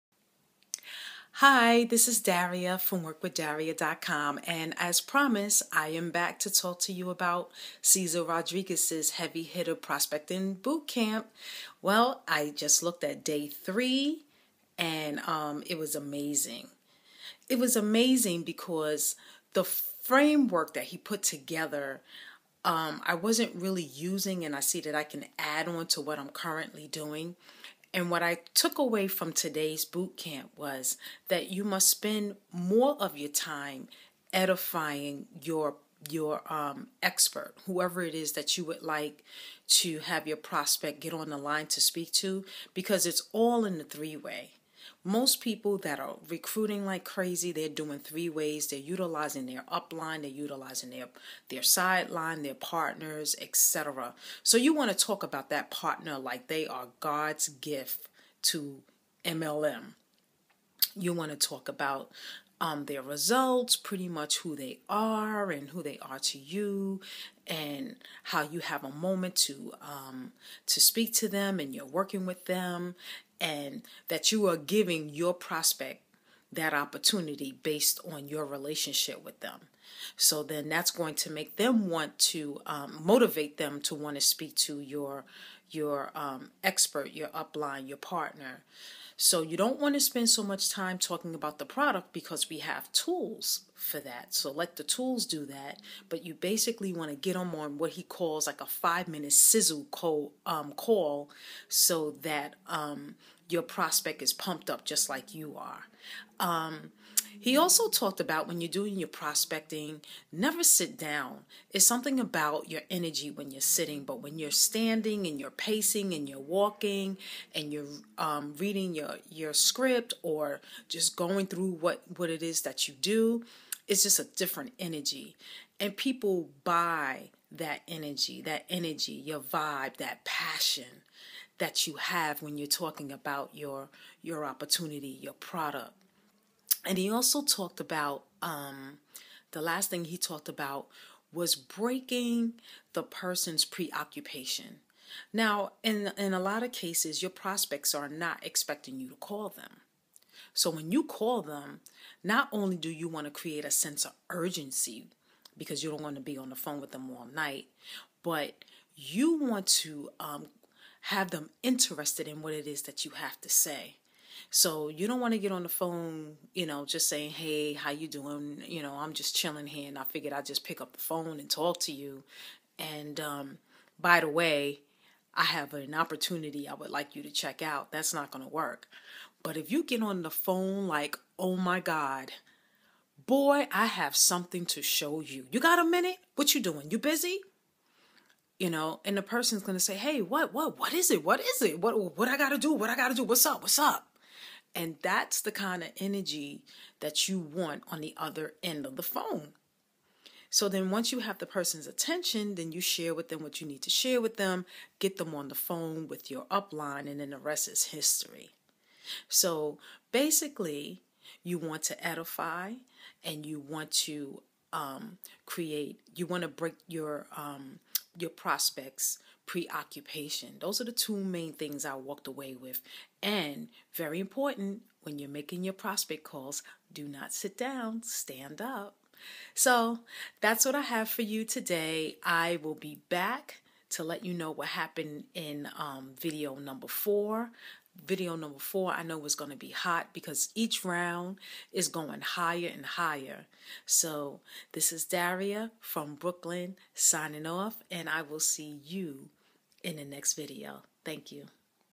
MLM | How to Get Your Prospects Interested in your Opportunity
How to get your prospects interested in your opportunity? It's not that difficult. You just have to be aware of some things. Do you have that emotional edge when talking to your prospects? People are attracted to your music, vibe or passion. See how to break someone's preoccupation and get them interested in your opportunity. Watch out for the next video. It's going to be hot!